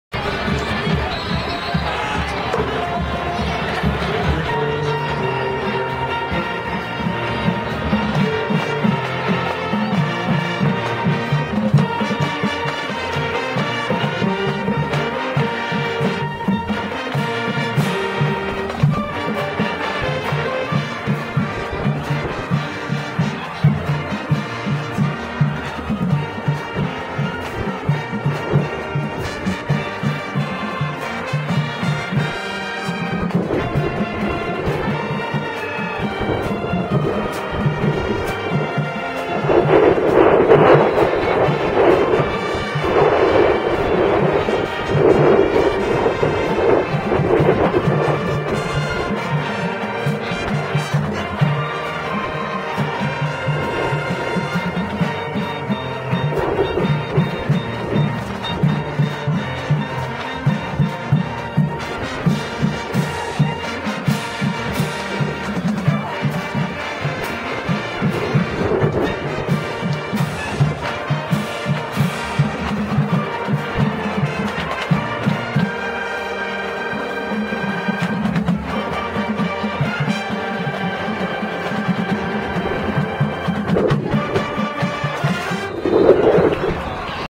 The John Jay Mustang Band performs at the football game vs Marshall (Senior Night) on November 9, 2012